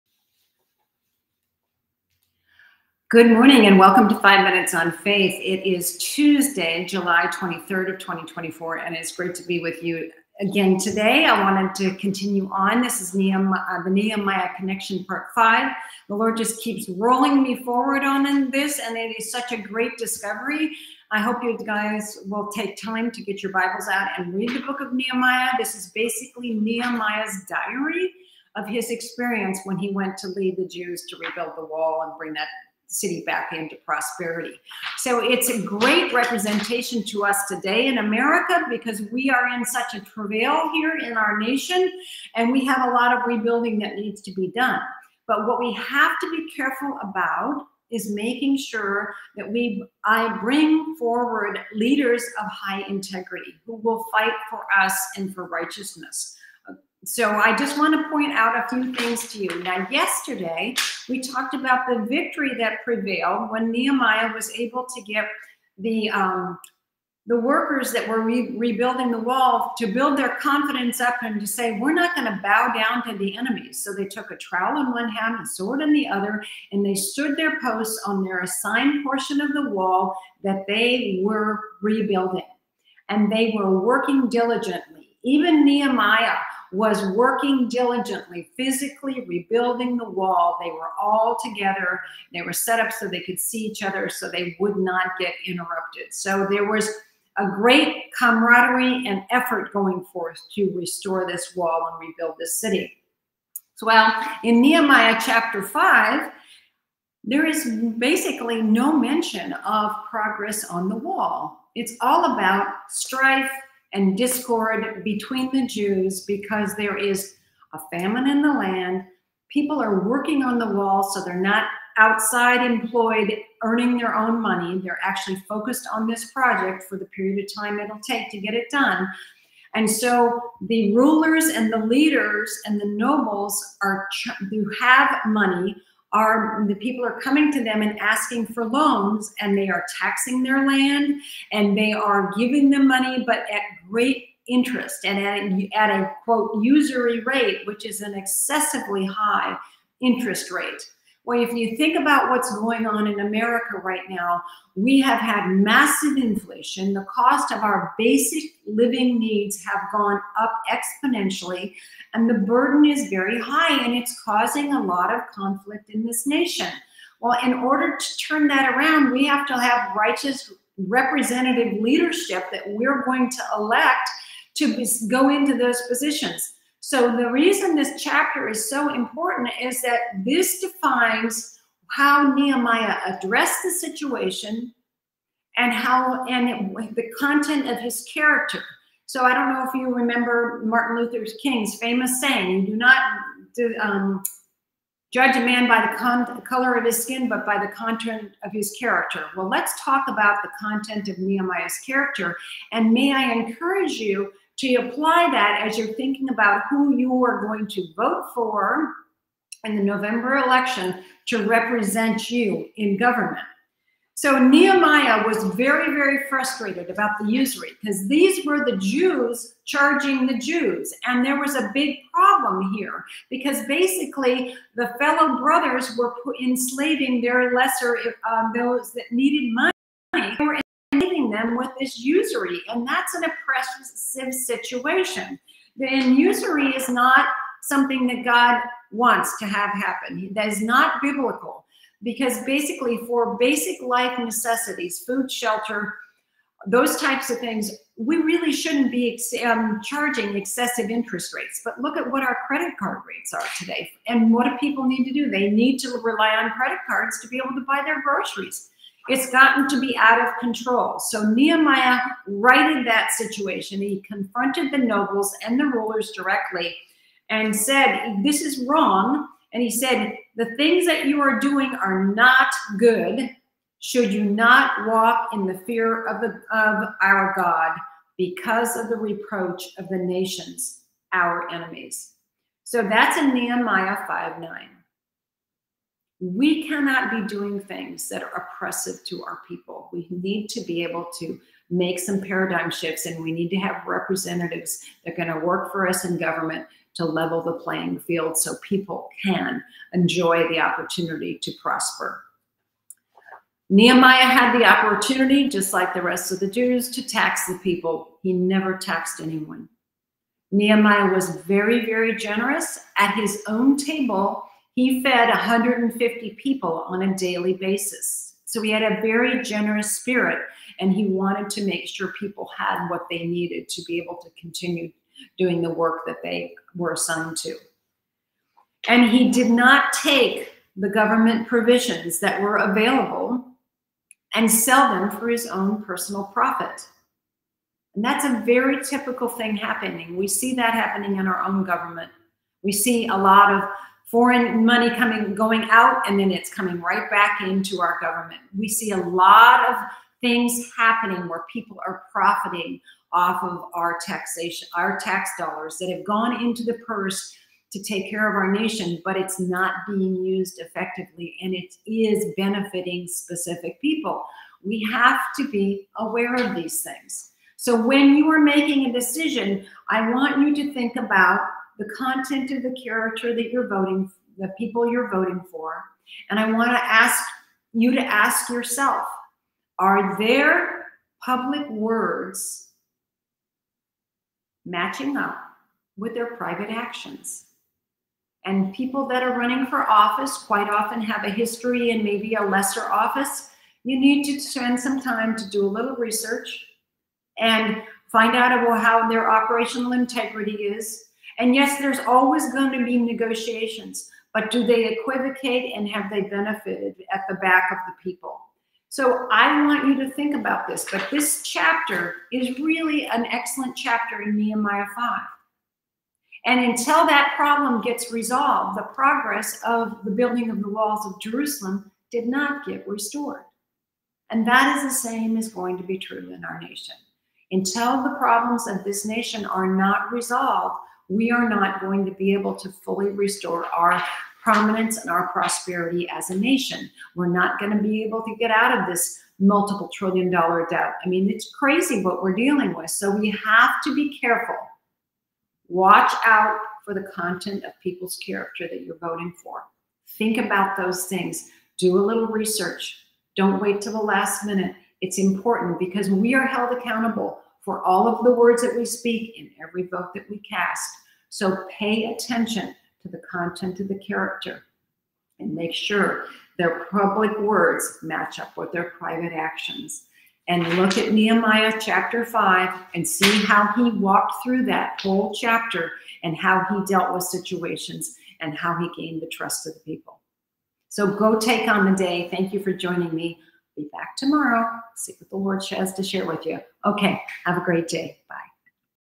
5 Minutes on Faith: The Nehemiah Connection Part 5 #103
Beware of Public Words and Private Actions!

Nehemiah 5: despite the great victory in Chapter 4 where the people are building the wall, with a sword in one hand and a trowel in the other, in Chapter 5 the people are in strife due to finances, because they are greatly oppressed by their fellow Jews with heavy taxation on their properties, famine and great inflation.    Sound familiar?

Nehemiah is angry with the usury (excessive interest that was basically enslaving fellow Jews) and the lack of unity of the people.

As a great leader Nehemiah confronted the nobles and rulers directly, reminding them it was wrong to have Jews sold into slavery because they could not pay off high interest loans for basic life necessities.

Nehemiah 5:9 “AGAIN I SAID, “THE THING WHICH YOU ARE DOING IS NOT GOOD; SHOULD YOU NOT WALK IN THE FEAR OF OUR GOD BECAUSE OF THE REPROACH OF THE NATIONS, OUR ENEMIES?”

Nehemiah did not tax the people.

Nehemiah was generous: feeding 150 Jews and rulers at his table every day.

Nehemiah did not take more of the government provisions and sell them to the Jews for profit.

Nehemiah’s public words reflected the truth of his private actions.

ABOUT

5 minutes of sharing insightful, encouraging messages on faith.